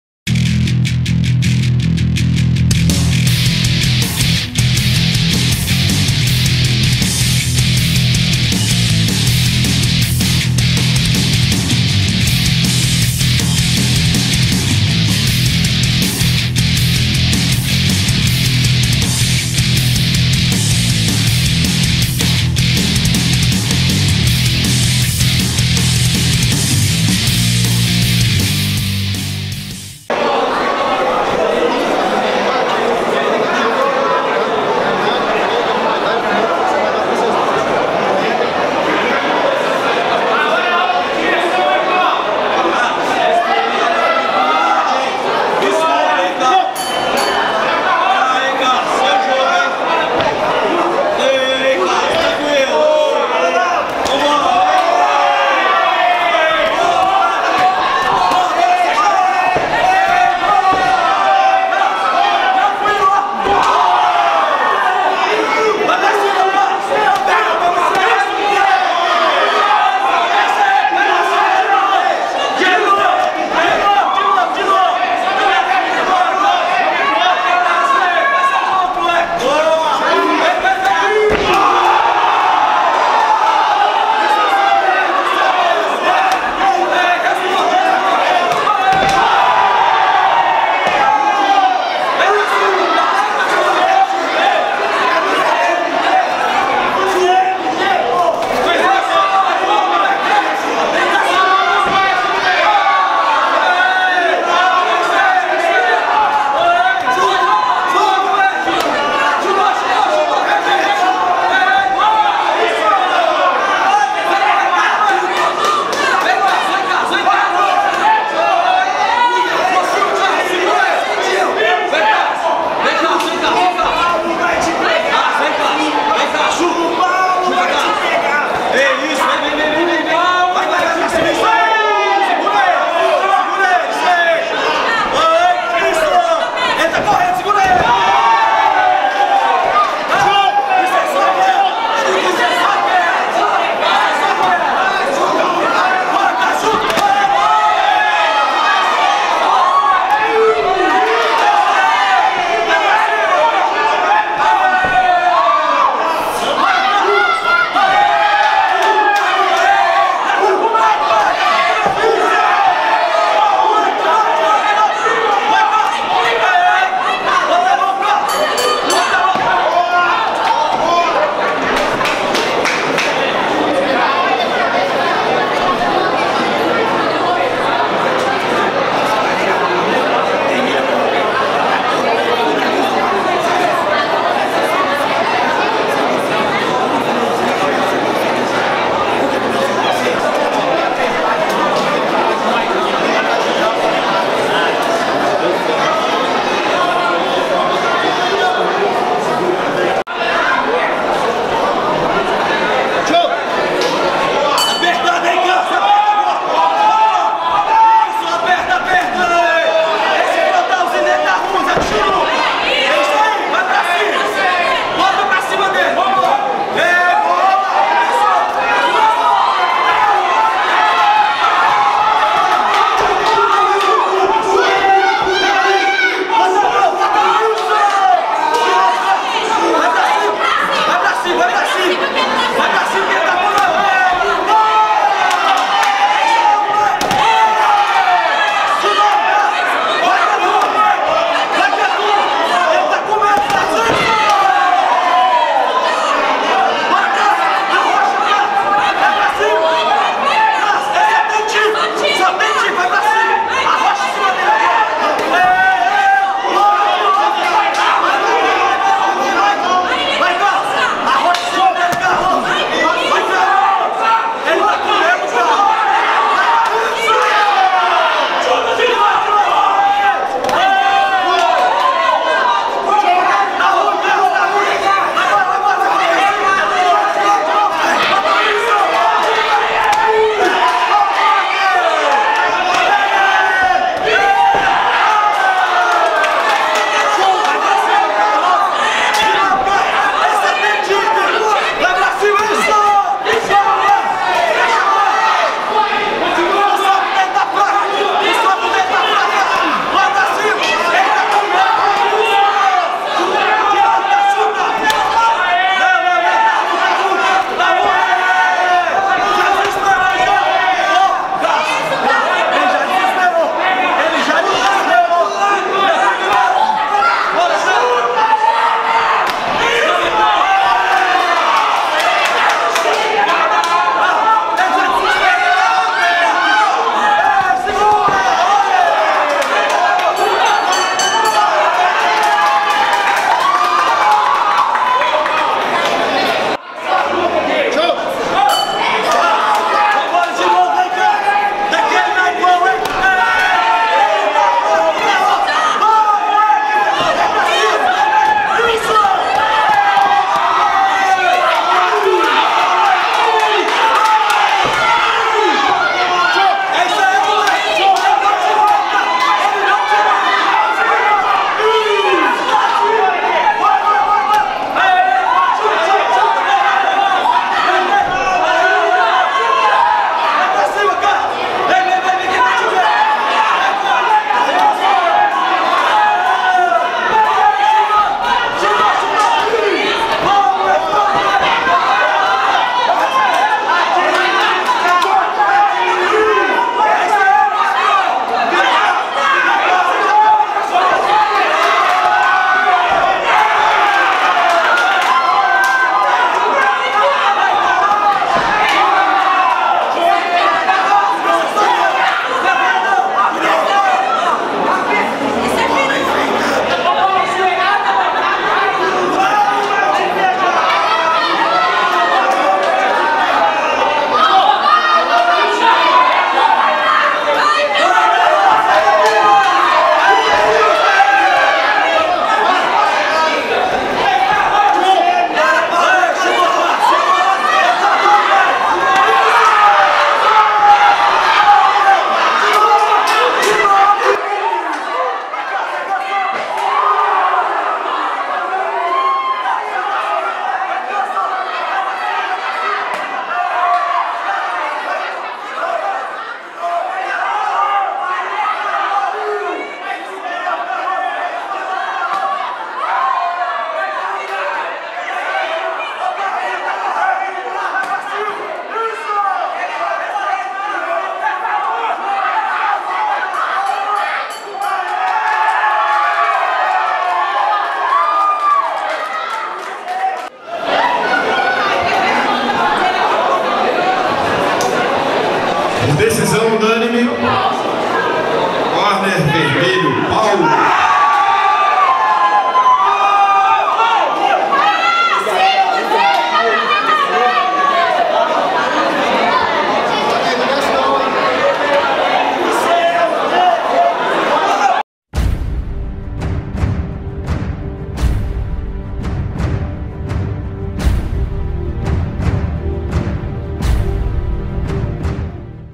Carlos vs Paulo Henrique - Contagem Fight Night 3
Paulo (Cabeça Muay Thai) venceu Carlos Henrique (CT Nocaute) por pontos – 61kg.